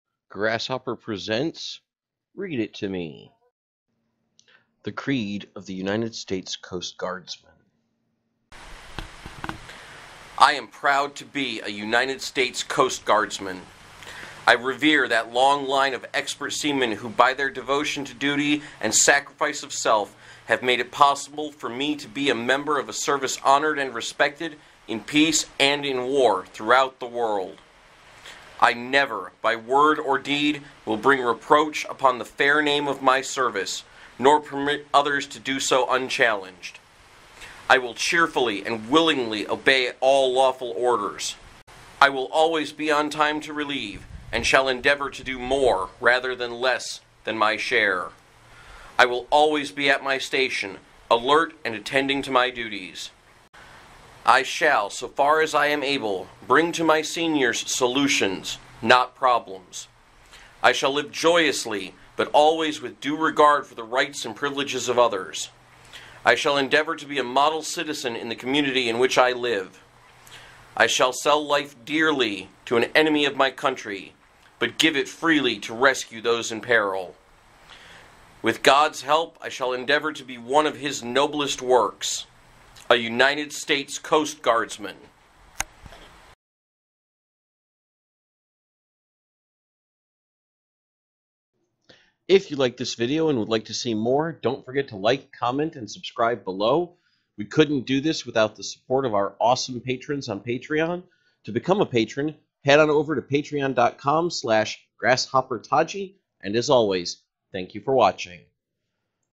The Creed of the United States Coast Guardsman || Grasshopper Presents || Read it to Me
Grasshopper presents:  Read it to Me, the Creed of the United States Coast Guardsman

You get what it says on the tin.